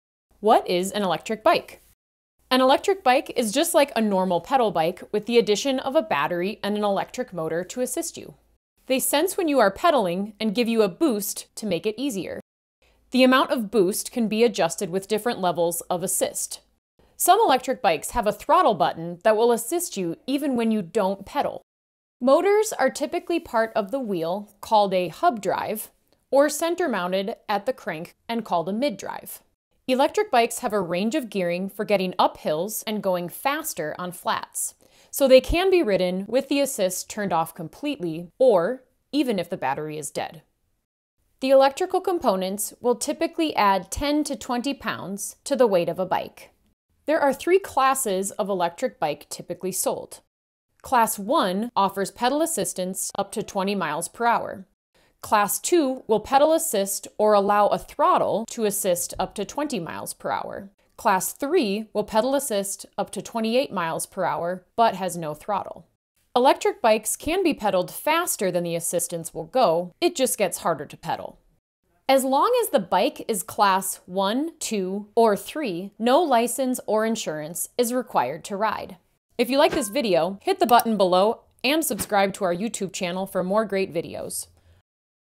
What is an Electric Bike?
Electric bikes look a lot like the non-electric bikes you’re familiar with, but they're equipped with a battery and a small electric motor to assist you in pedaling faster and farther. E-bikes are offered in a variety of styles, including cargo bikes, commuting bikes, mountain bikes, folding bikes and more.

The motors on an electric bike senses when you’re pedaling and gives you a boost to propel the bike forward, making pedaling easier. Many e-bikes have several levels (or modes) of assistance that allow you to adjust the amount of boost you get. Some electric bikes provide a boost only when you pedal, others have a throttle button that will assist you even when you don’t.

E-bikes are categorized into three main classes, which often determine where you can ride them. Most bike manufacturers and state, local and other entities have adopted this system, which defines e-bikes as a low-speed bicycle with fully operational pedals and an electric motor of less than 750 watts (about 1 horsepower).

An REI Co-op media Production-
Creative Producer/Content/Filmmaking: Jason Lohr-Johnson
Talent and VoiceOver: Kat Parks
Editor: Katie Van Fleet
Executive Producers: Joe Crosby and Alissa Hudson